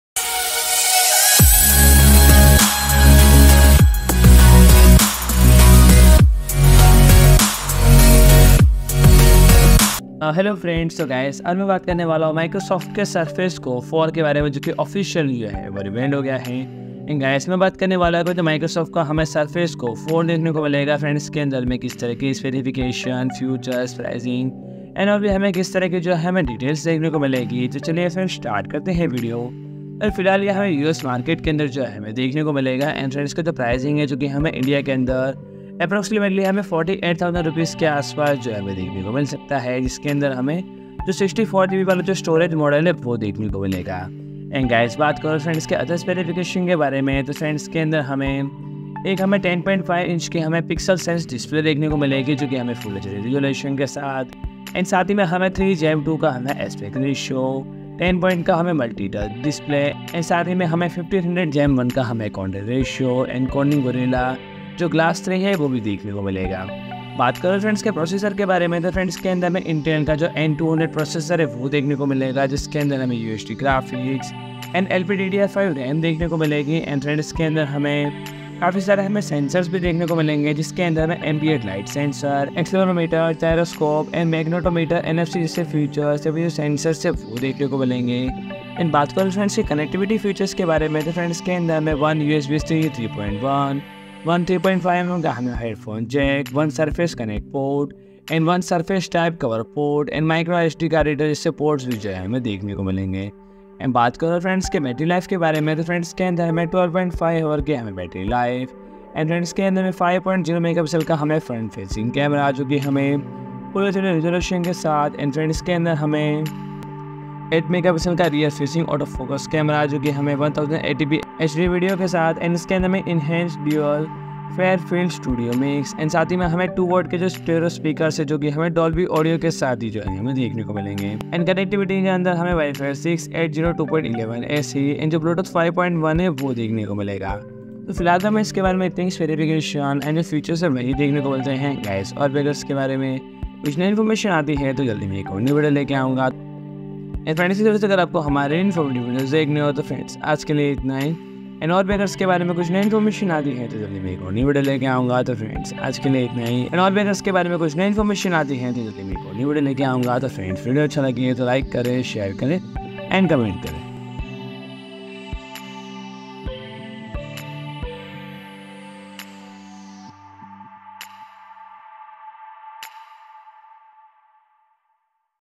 Microsoft Surface Go 4 Launched With 10.5″ FHD Touch Display, Intel N200 Processor & More Details
PRODUCT BRAND STORE AMAZON

It still has a 10.5-inch PixelSense 3:2 aspect touchscreen display with adjustable kickstand that enables the laptop to be used in multiple form factors. This replaces the 10th Gen Intel Core processor with an Intel 200 processor, and the company says that it delivers up to 4.5 times more performance than the original Surface Go, with a longer 12.5-hour battery life. The Surface Go 4 is one of the few Windows 11 PCs that qualifies for Microsoft 365 for frontline, and now has integrated NFC that can be accessed securely and easily with a compatible NFC security key, like a YubiKey. Microsoft has removed the 4GB model, so it now comes with 8GB LPDDR5 RAM and gets faster UFS storage up to 256GB.